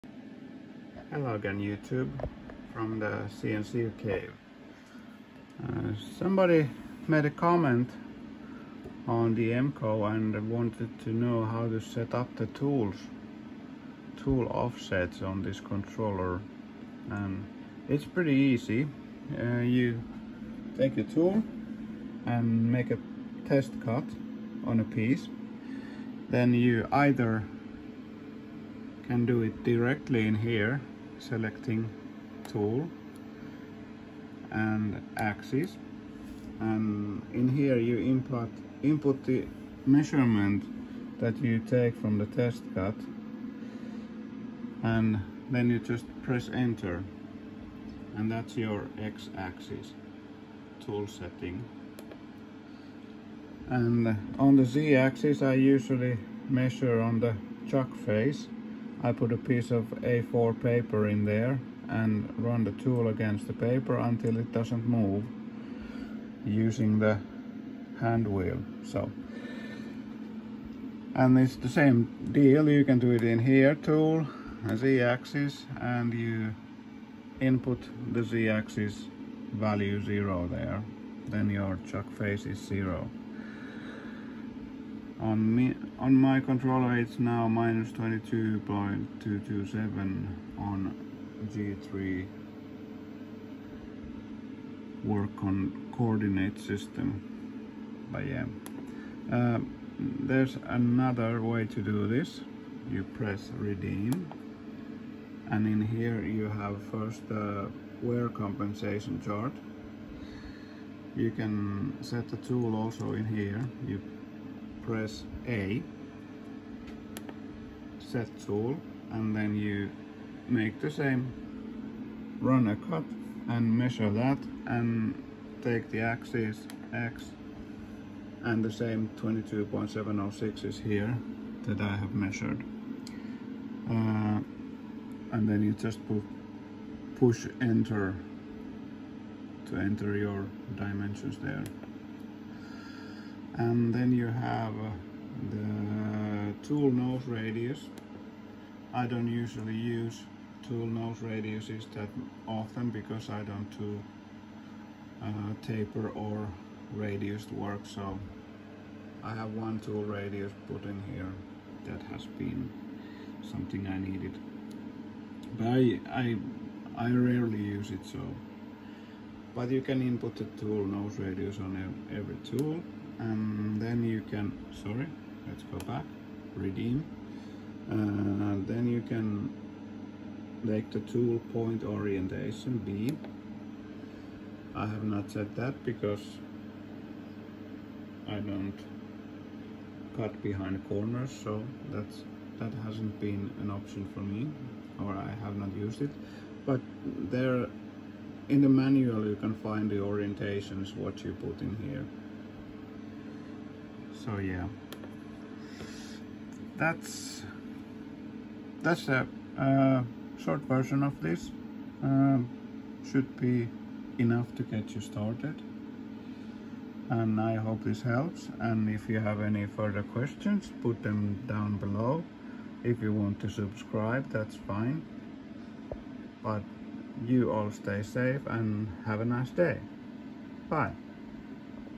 SZGH-990T Tool offsets
Short how to set tool offsets on the Szgh-990Tdb controller.